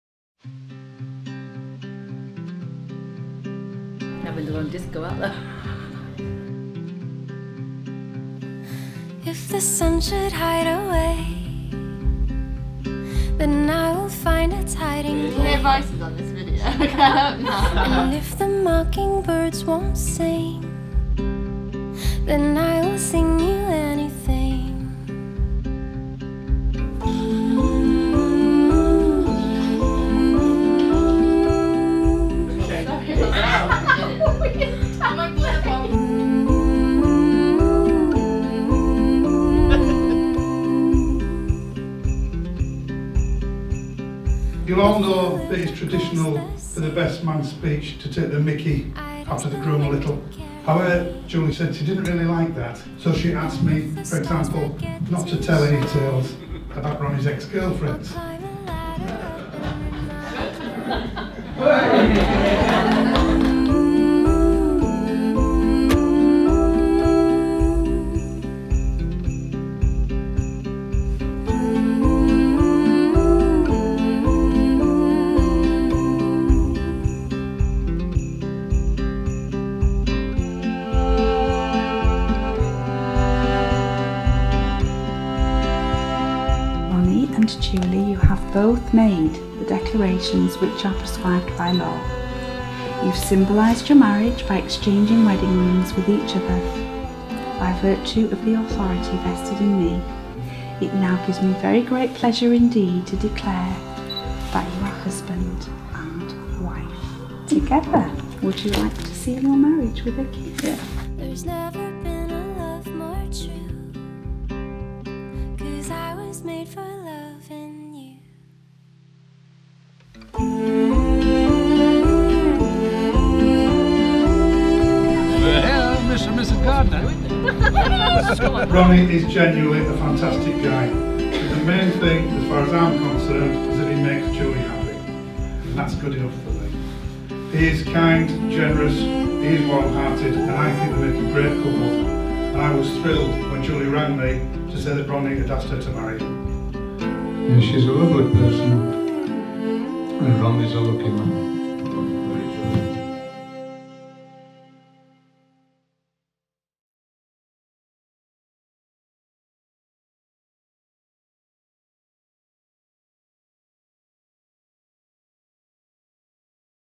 Julie and Ron | Fun Scarborough Wedding Trailer Film
Julie and Ron's day began with the bridal preparations, before heading to Scarborough Registry Office in a vintage car for the ceremony.  From there we took a double-decker bus to the Golf Club for the reception.